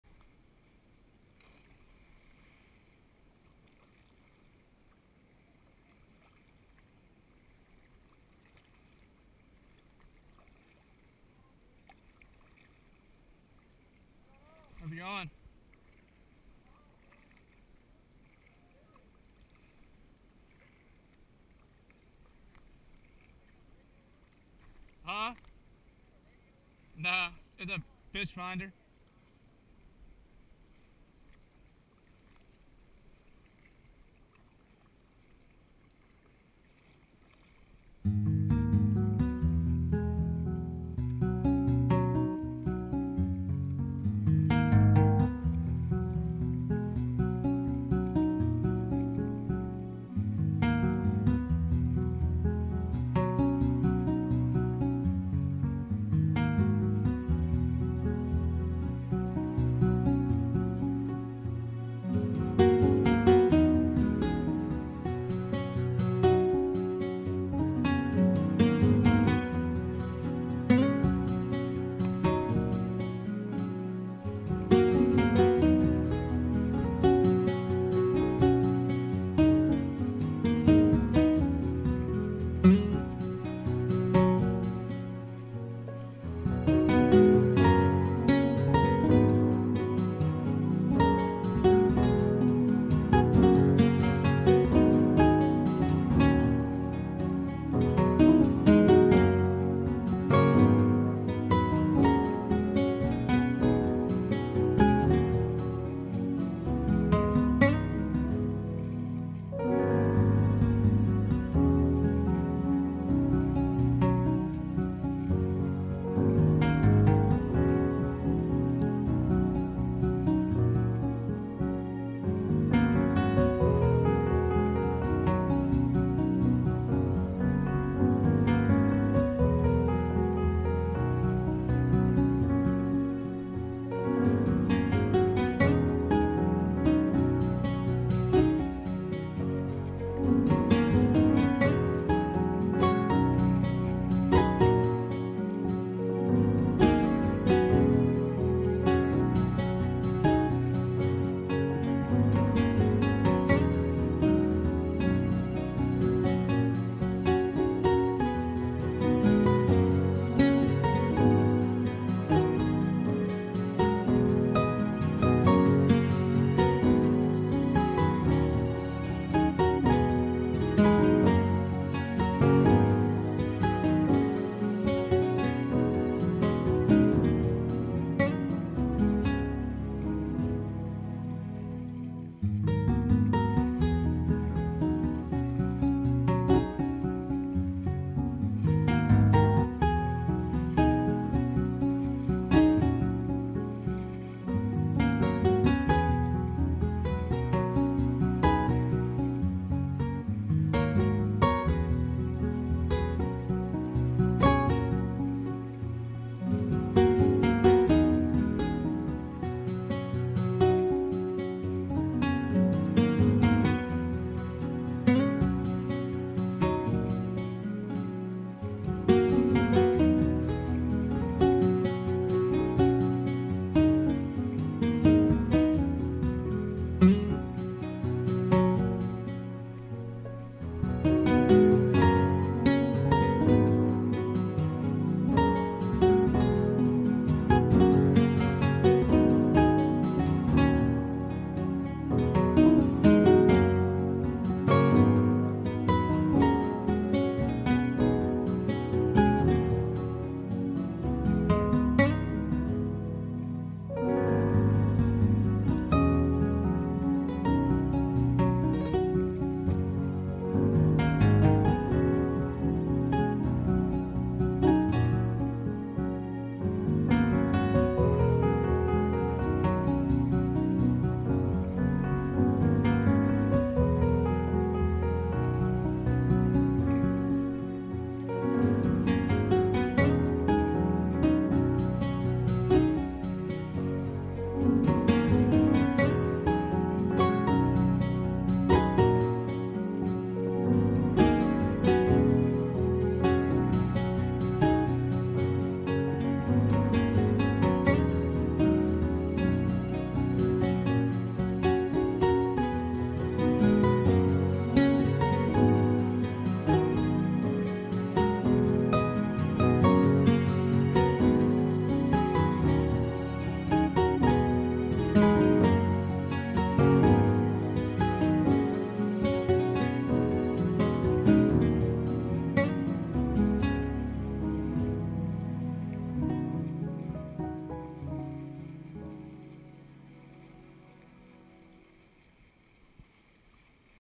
GoPro HD Paddling By The Cliff Jumpers
Paddling by the cliffs at Percy Priest Lake outside of Nashville, TN, saw some kids cliff jumping. Good thing I wasn't trolling a crank bait!